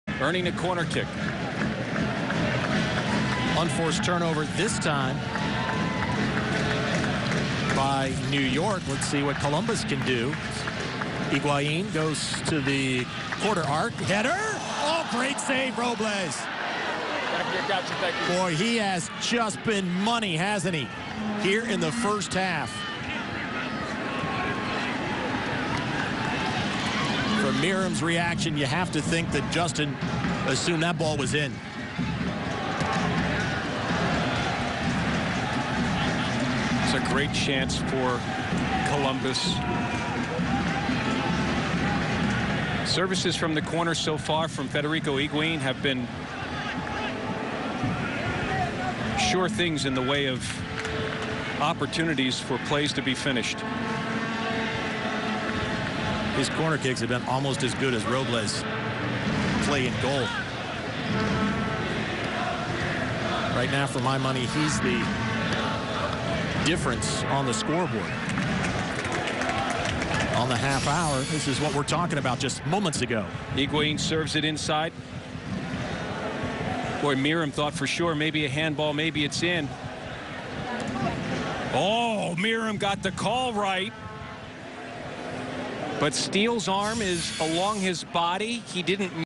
PRO: Play of the Week 24 - Columbus Crew vs. New York Red Bulls
Week 24's Play of the week sees PRO Training & Development Manager Paul Rejer look at three incidents, one coming from the recent game between Columbus Crew and New York Red Bulls